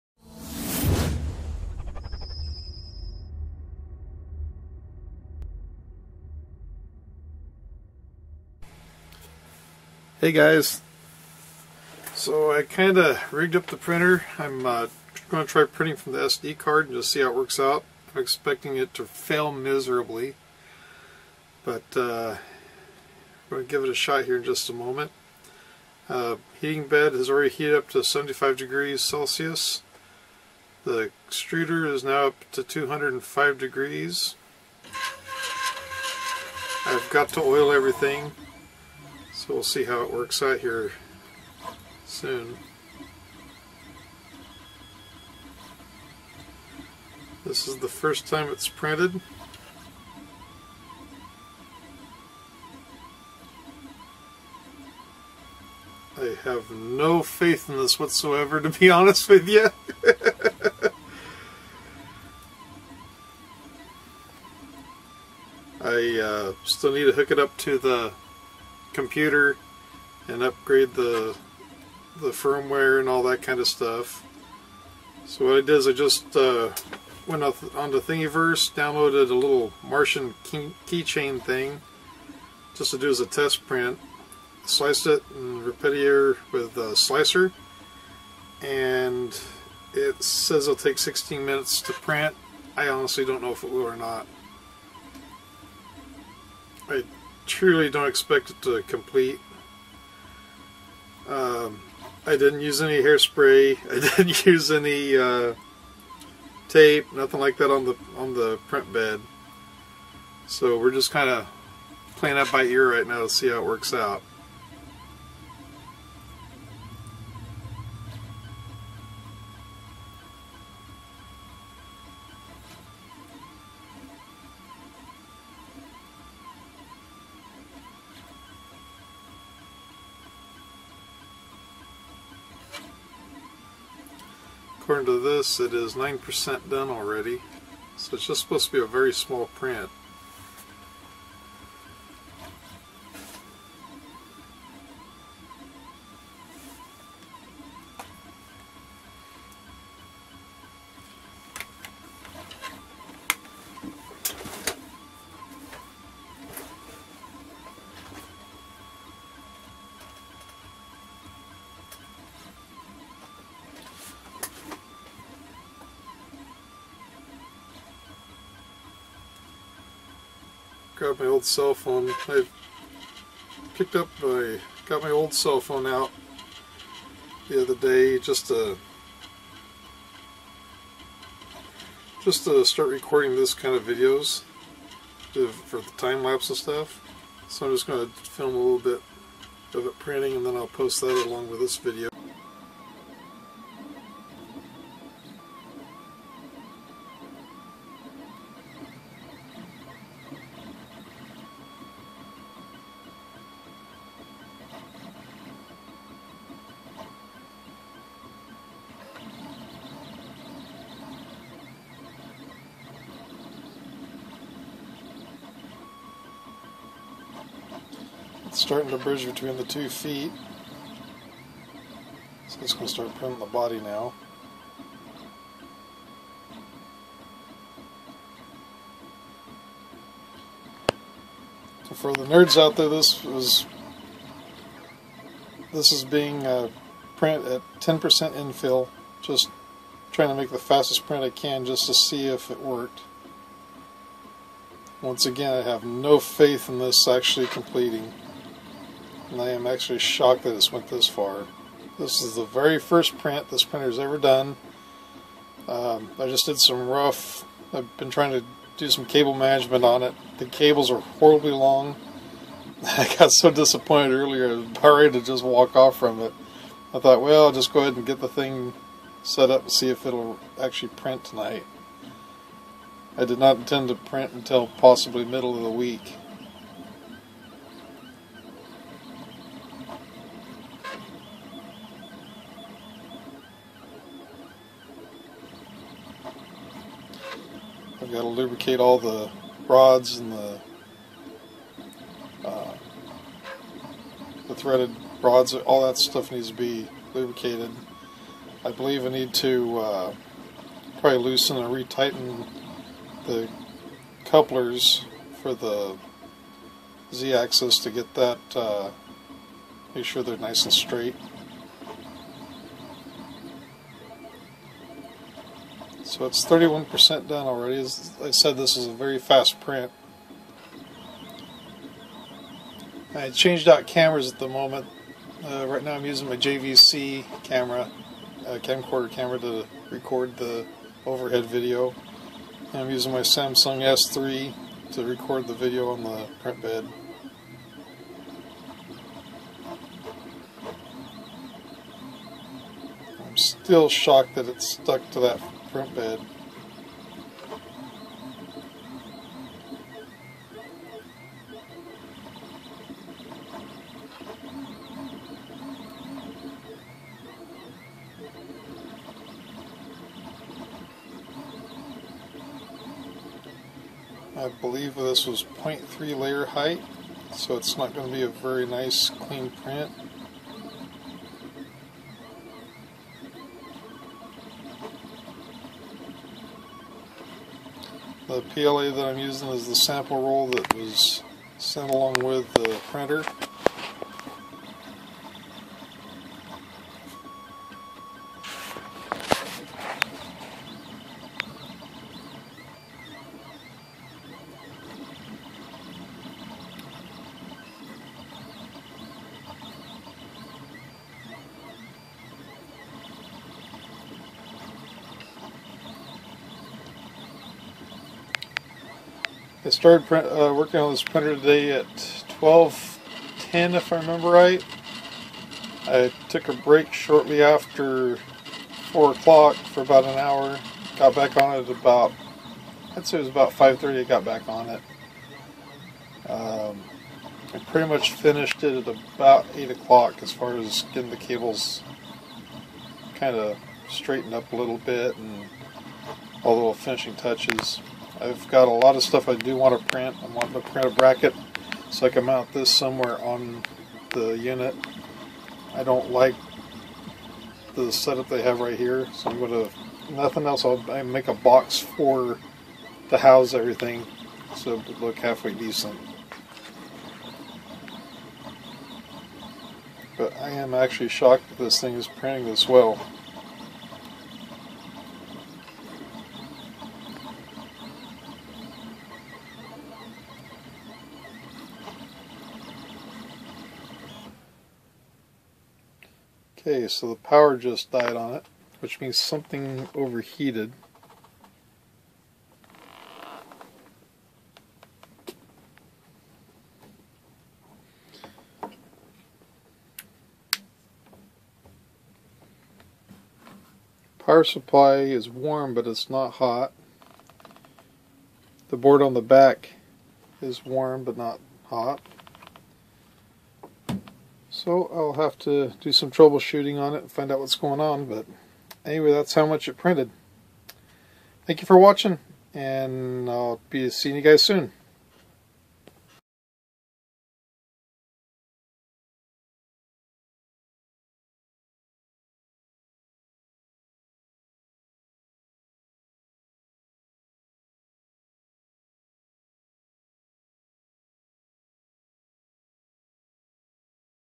Geeetech Aluminum Prusa I3 The First Print
This was the first print on the just built Geeetech Aluminum Prusa I3.  There had been no tweaks, or adjustments made other than leveling the bed and setting the z axis end stop.  I literally just downloaded Repetier and sliced the first thing that I could find on Thingiverse.  A Marvin Key Chain,
I didn't even set any of the specific settings before slicing.

The printer powered off unexpectedly.  It turned out that the power cord wasn't plugged in tight enough and it vibrated loose.

After 4-5 hours of building, it was nice to see it come to life.  I love my printer!!!